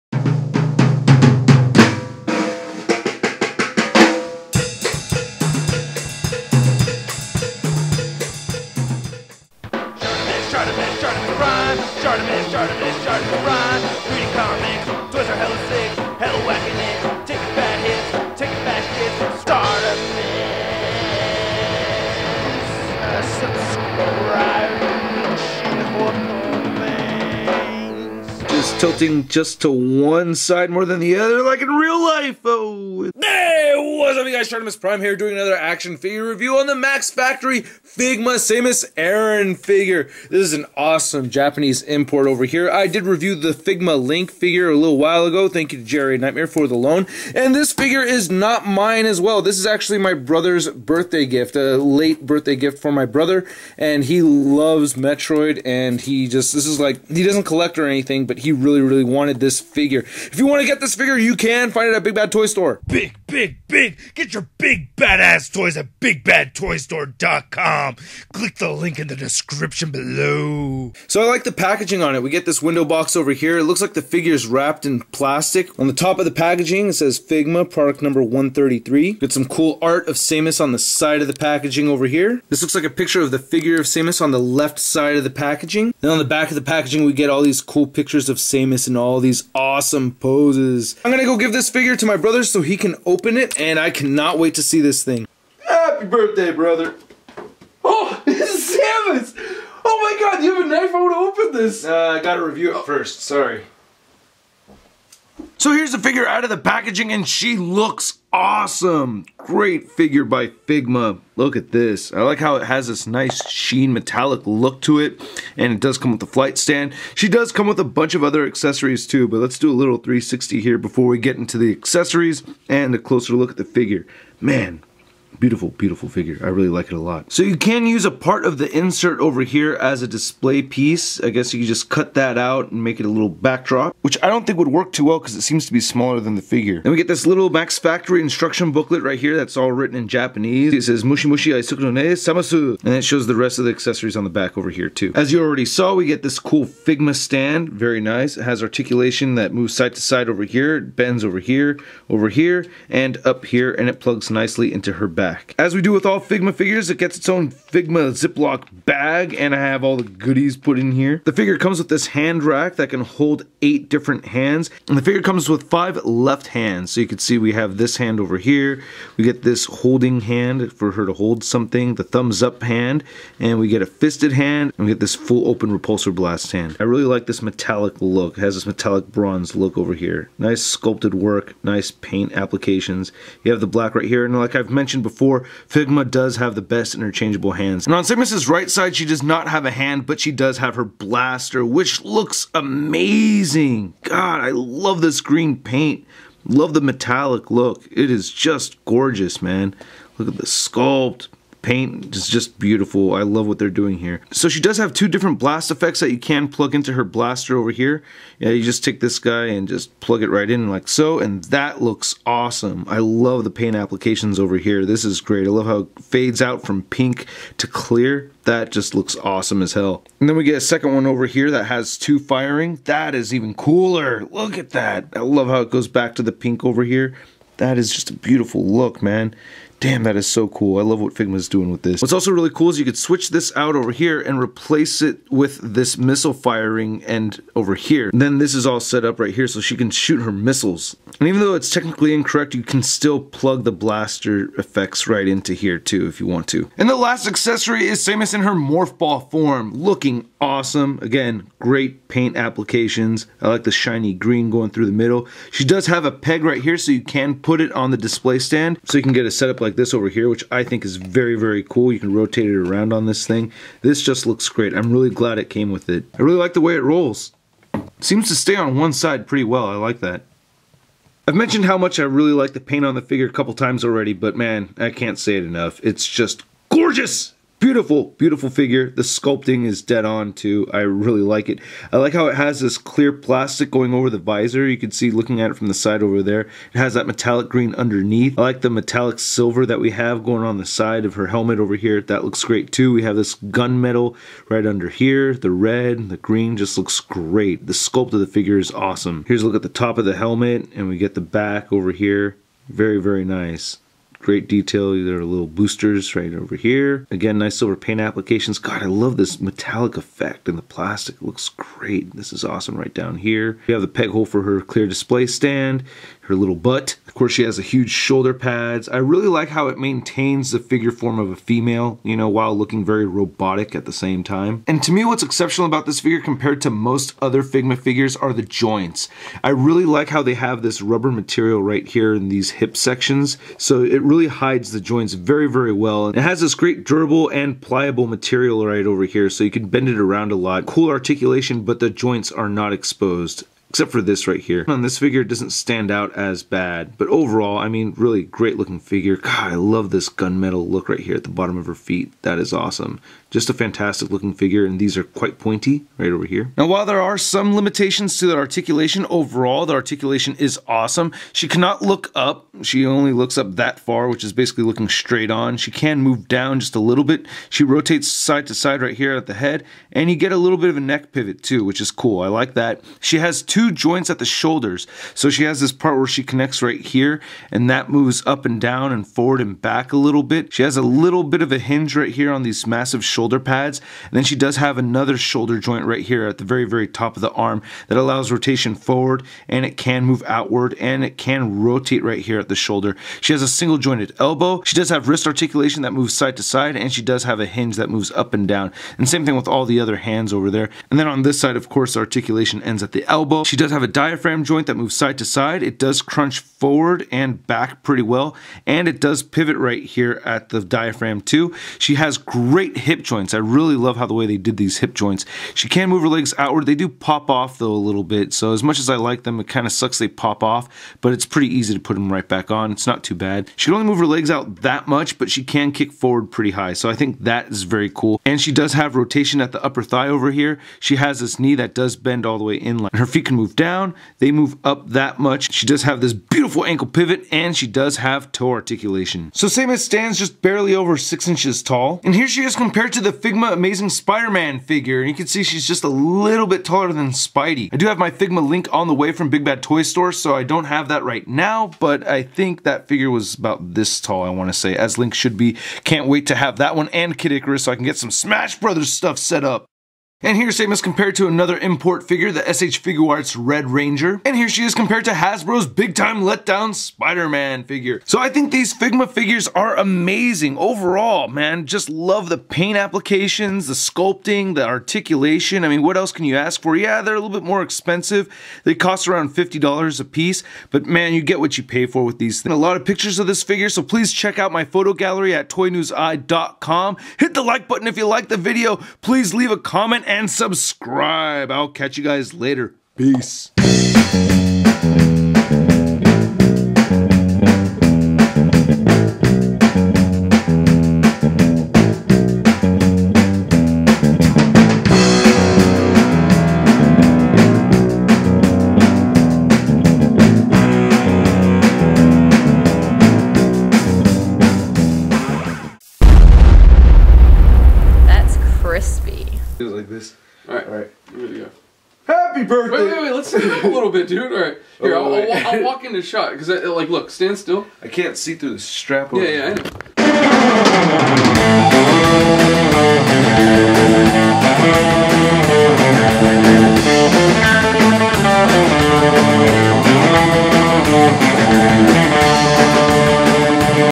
Figma Samus Aran Metroid Other M Nintendo Video Game Figure Review
Figure poses by my brother, Gordotron.